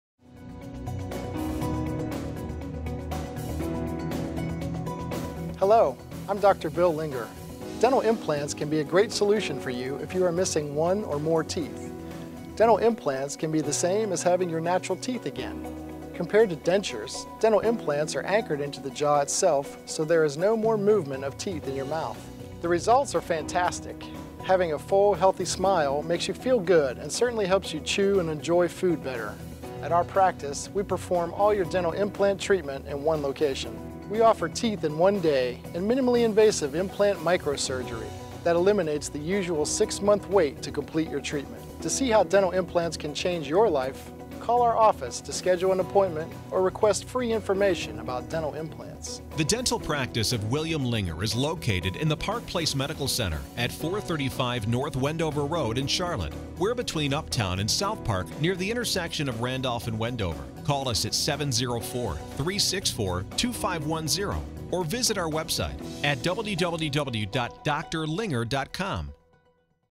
Dr William Linger DDS, MAGD Dental Implants
Charlotte's Premier Dental Implant Doctor provides everything you need for dental implants in one location.